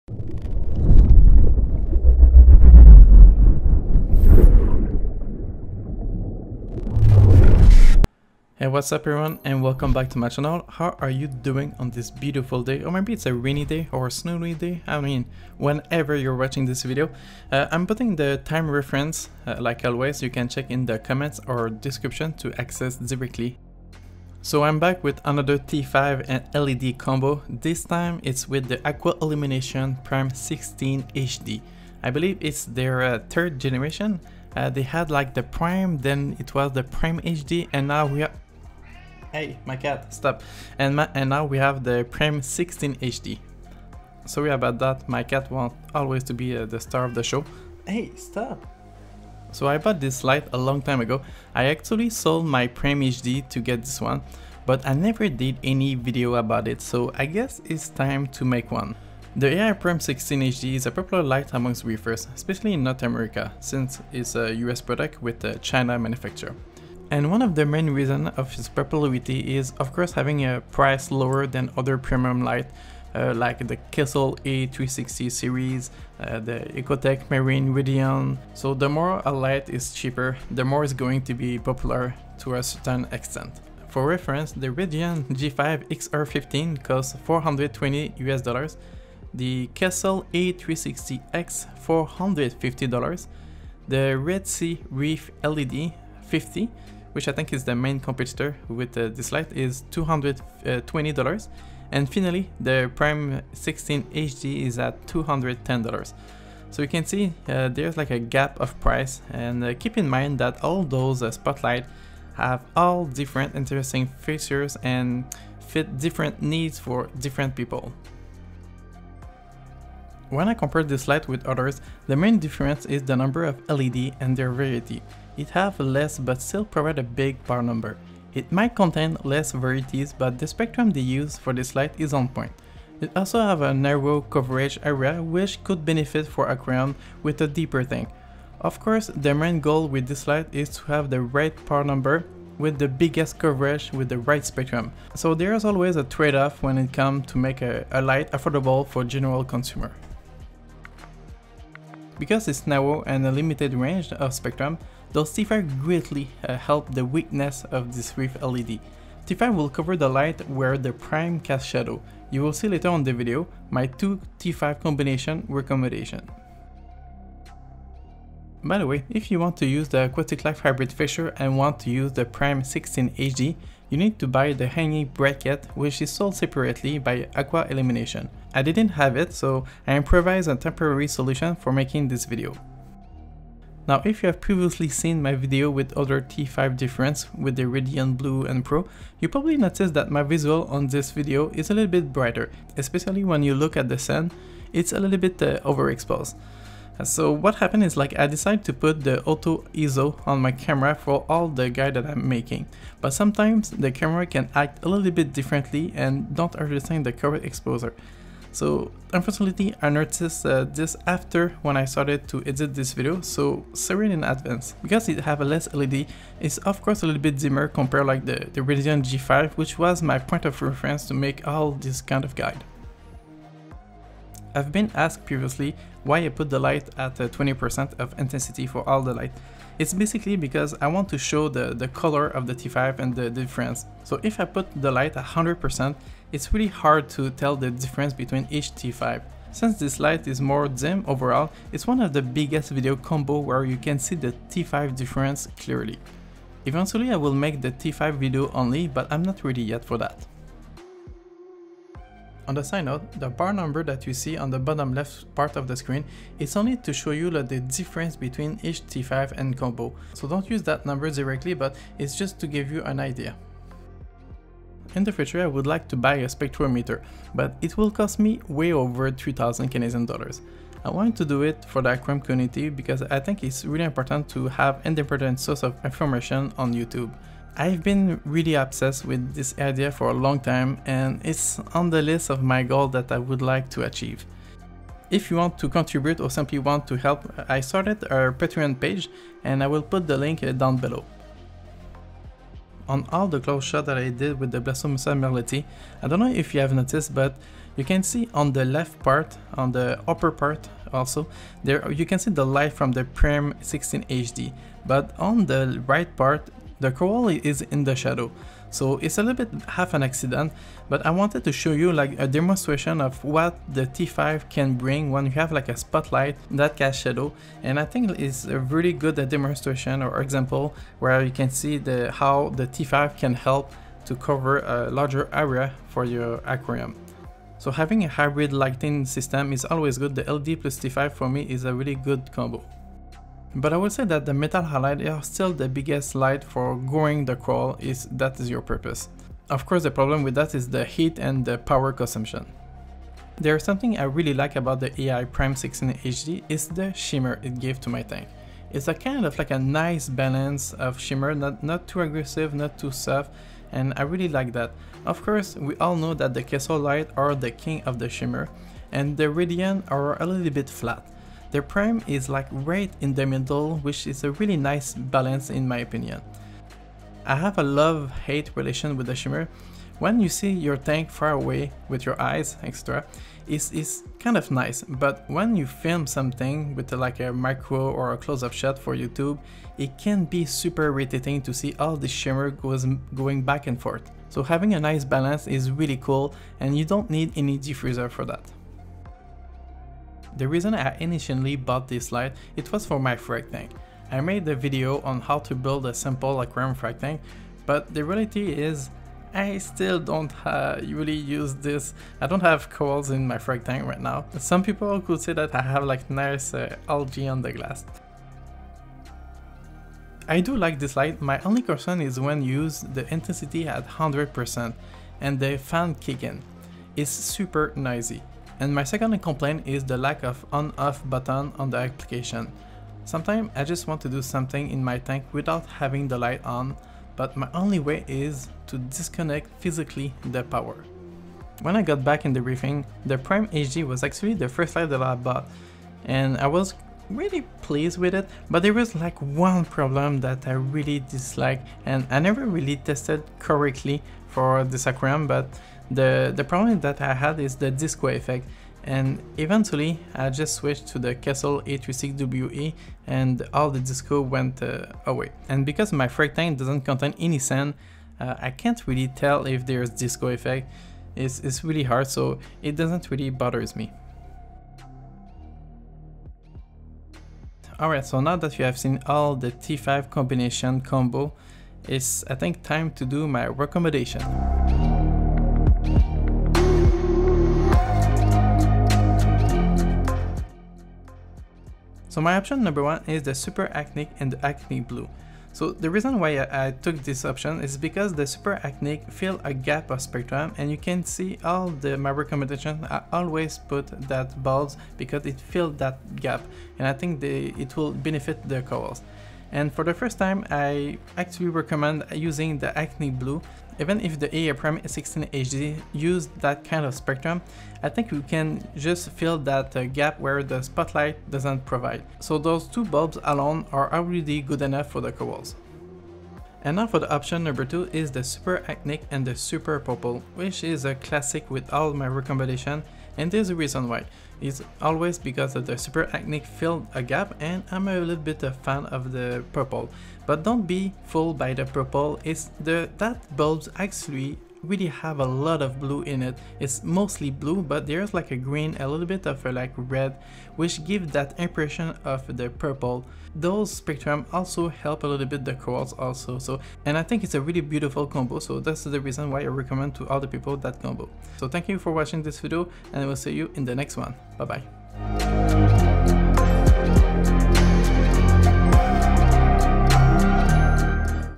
AI Prime 16 HD with T5 Combination
TIMESTAMP:
T5 Solo: 00:23
T5 Super Actinic: 01:40
T5 Blue Actinic: 02:56
T5 Super Purple: 04:12
T5 Aquablue Coral: 05:28
T5 Aquablue Azure: 06:44
T5 recommendation: 08:00

With the Prime 16HD from Aqua Illumination
All LED Active at 20%

The PAR number is only to show the difference between each T5, the number by itself is irrelevant.
The Spectrum is what I think we should pay attention to.

Thank you to Giesemann for sending me their T5 a long time ago and being patient ;)

Doing all this combination and Data took me a long time so any support of all kind is always welcome :)
Special shootout to Quarantine for helping me make this video.